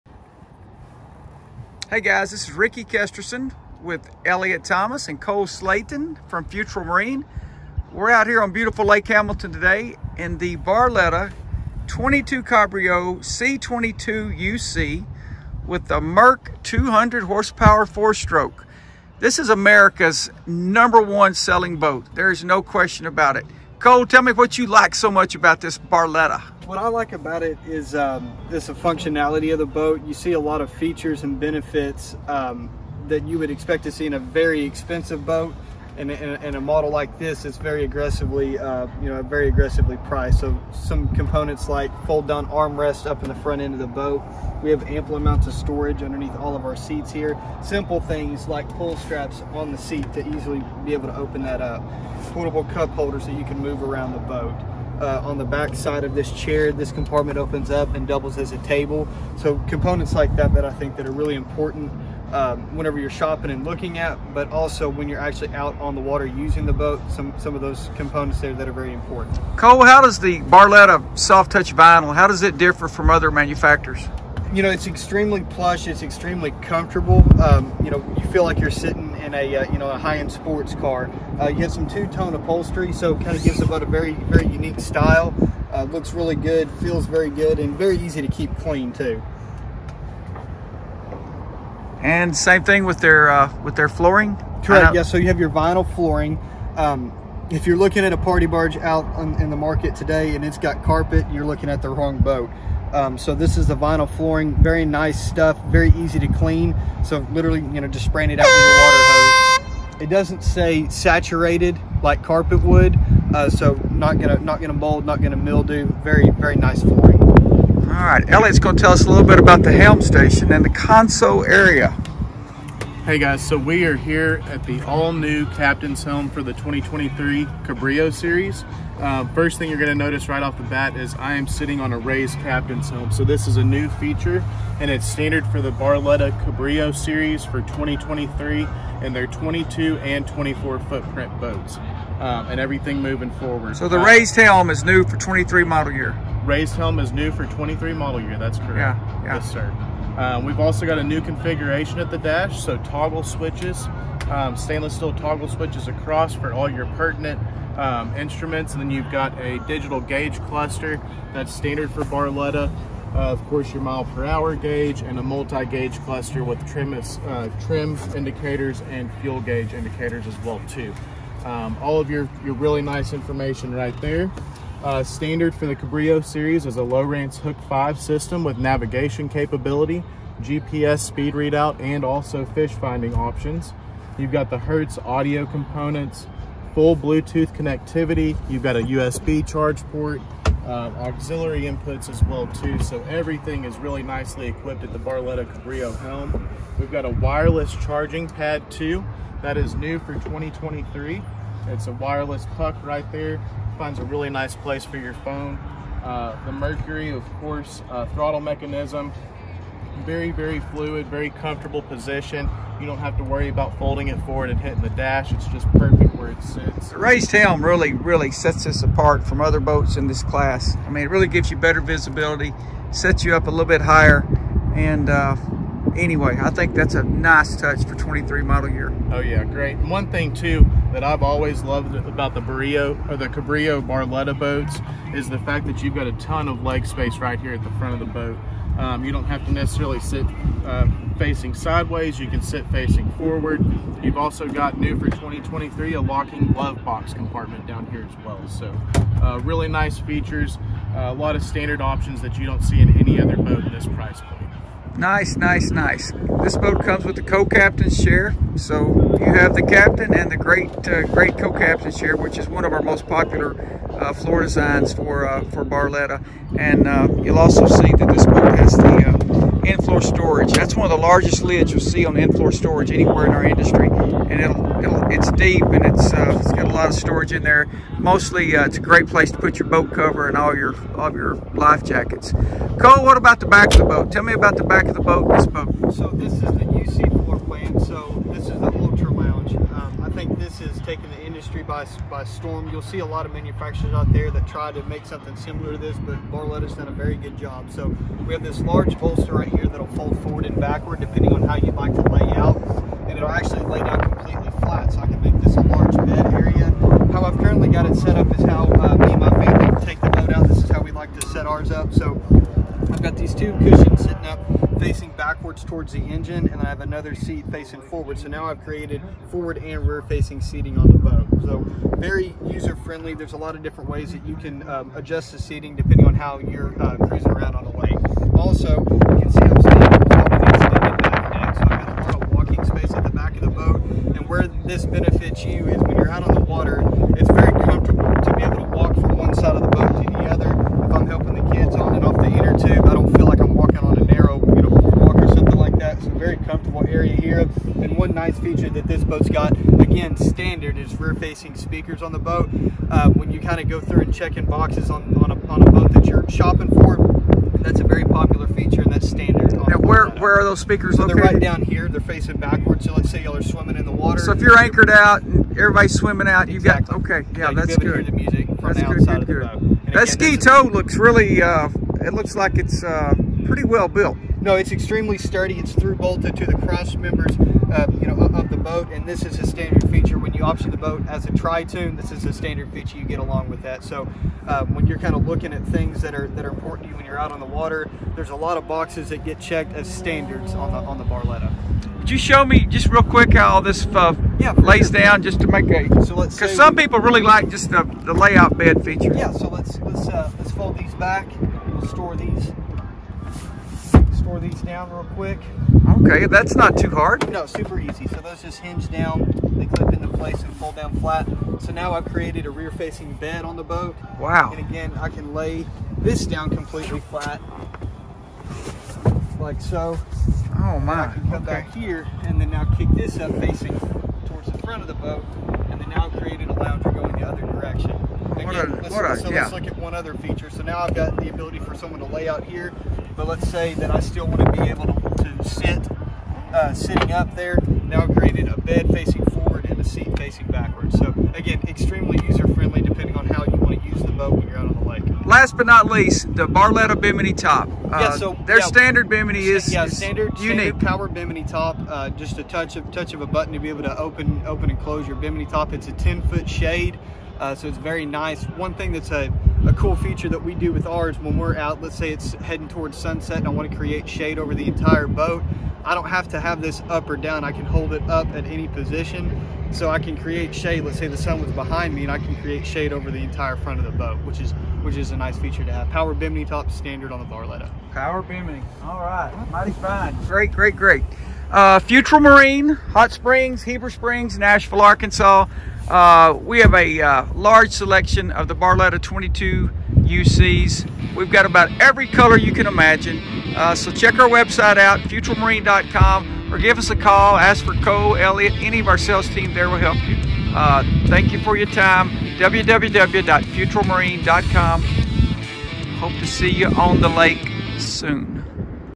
2023 Barletta Cabrio C22UC. On The Water. Features and Benefits. 2023's Best Selling Pontoon.
See the new 2023 model Barletta Cabrio C22UC on the water. This is the best selling pontoon of 2023. Incredible value. See all the new features and benefits. Answers to all your questions